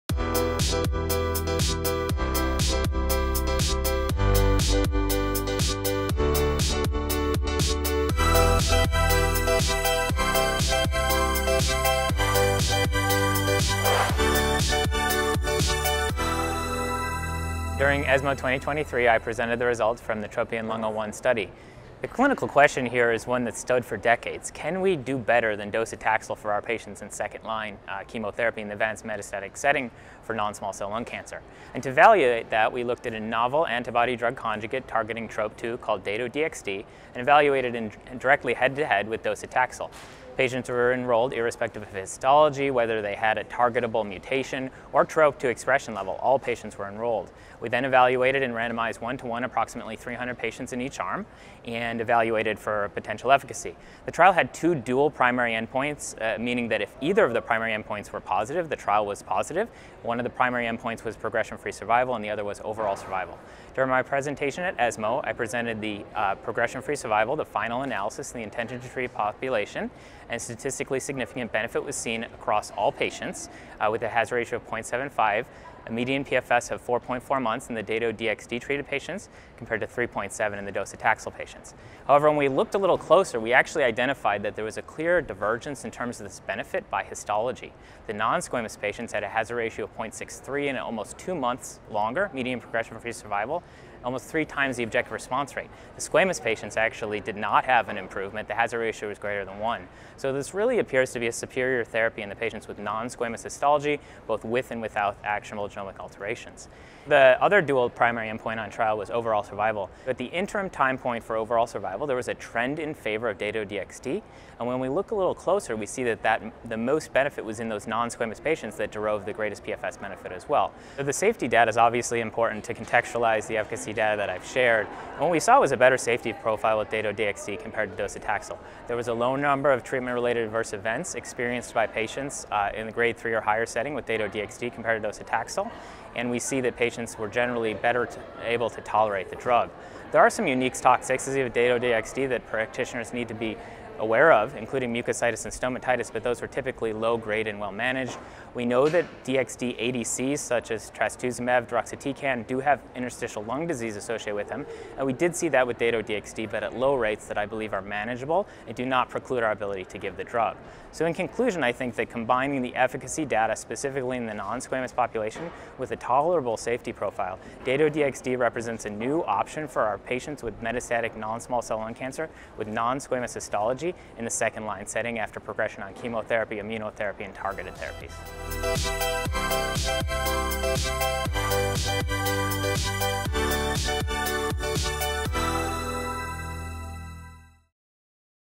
Highlights on datopotamab deruxtecan in advanced NSCLC: The TROPION-Lung01 study
Dr Aaron E. Lisberg reports on key results from ESMO Congress 2023 on LBA12 - Datopotamab deruxtecan (Dato-DXd) vs docetaxel in previously treated advanced/metastatic (adv/met) non-small cell lung cancer (NSCLC): results of the randomized phase 3 study TROPION-Lung01